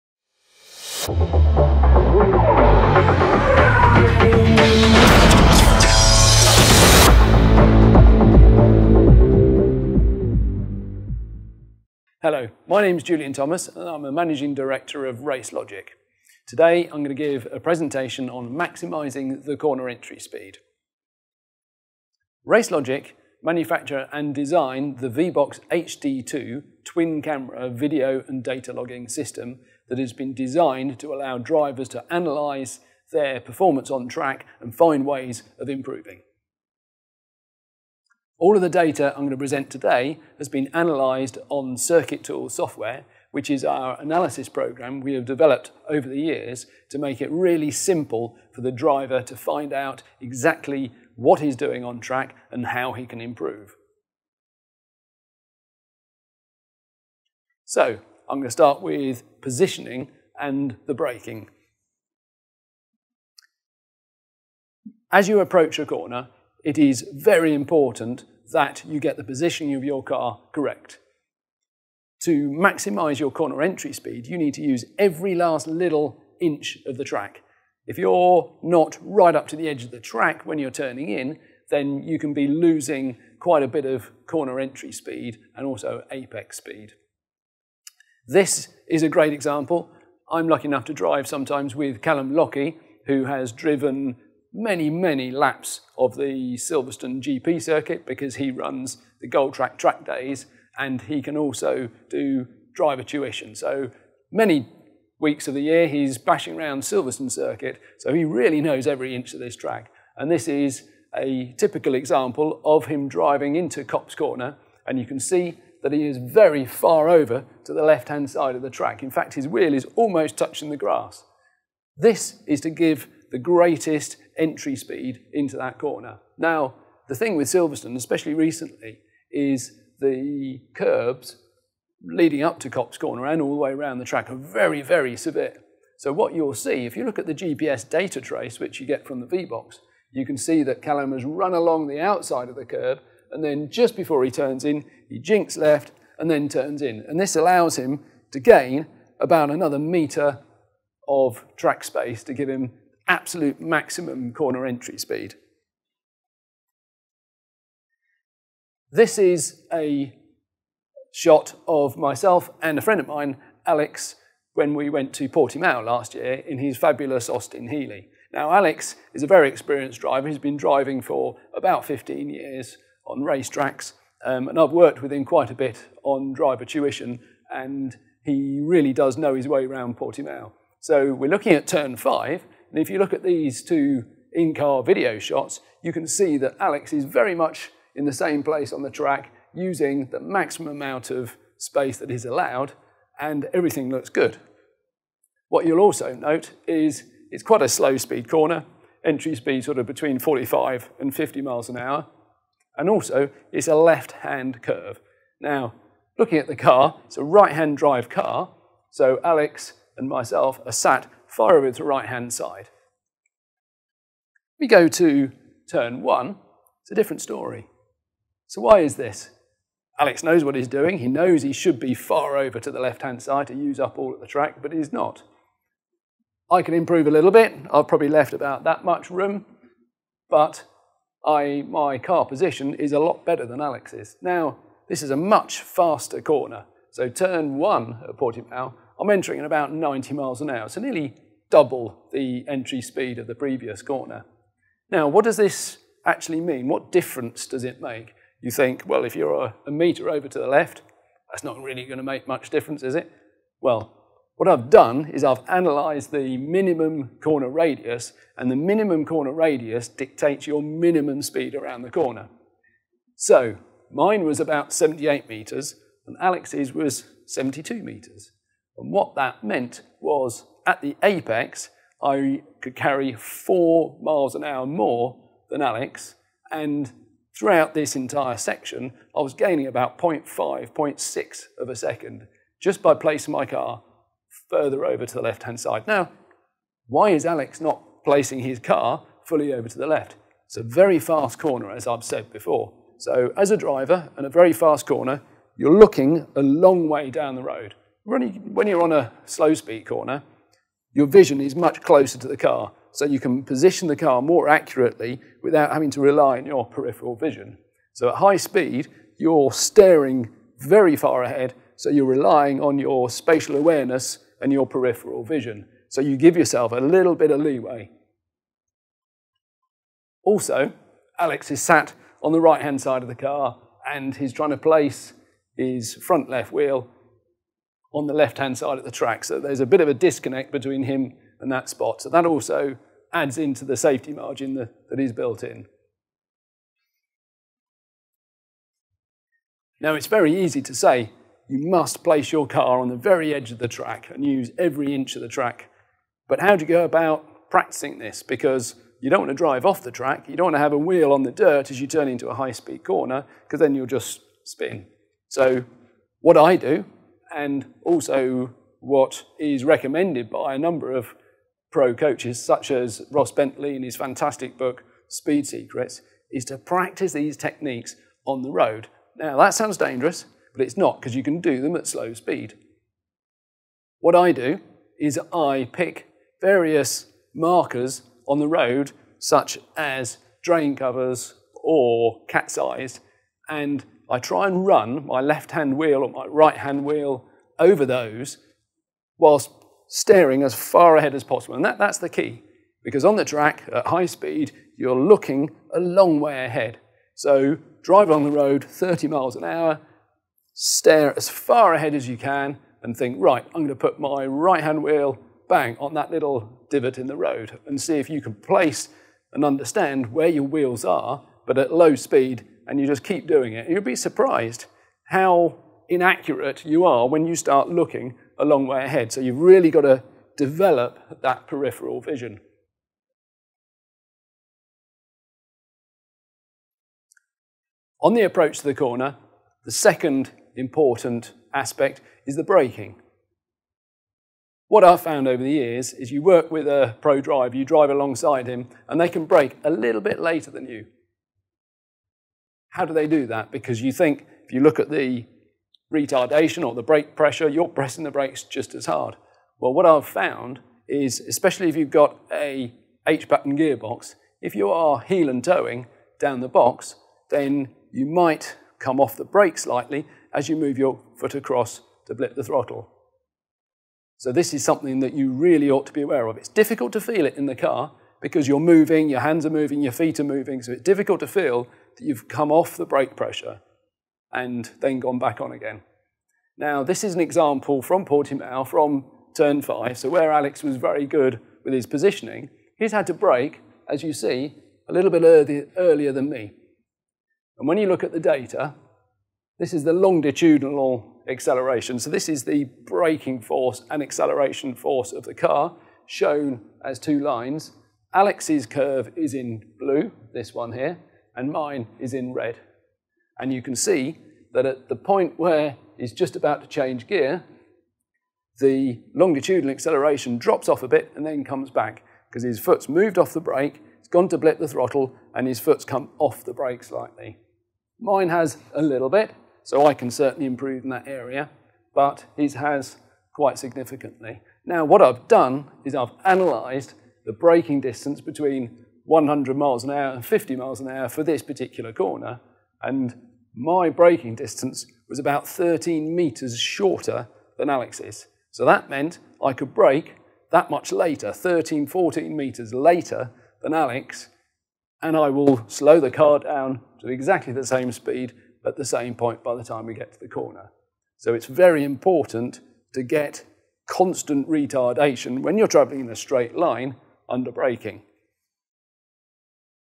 Mastering Corner Entry - Part 1 - Positioning and Braking
As you approach a corner it is very important that you get the positioning of your vehicle correct. To maximise your corner entry speed you need to use every inch of the track. If you are not right up to the edge of the track when turning in, you can be losing quite a lot of corner entry and apex speed.